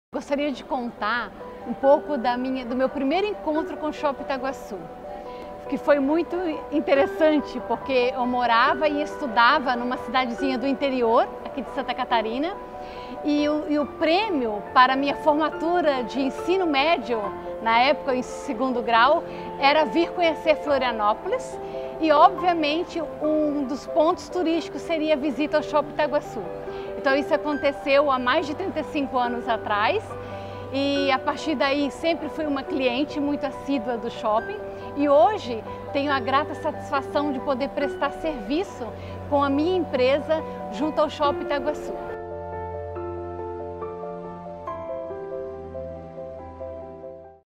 Shopping Itaguaçu 40 anos - Edineia Romão
Olha que legal!! A Edineia nos visita desde a infância e após anos ela se tornou produtora, e hoje faz diversos eventos LINDOS em colaboração com o Shopping Itaguaçu ♥

Você tem uma história semelhante a essa nesses 40 anos? Conte aqui nos comentários pra gente!!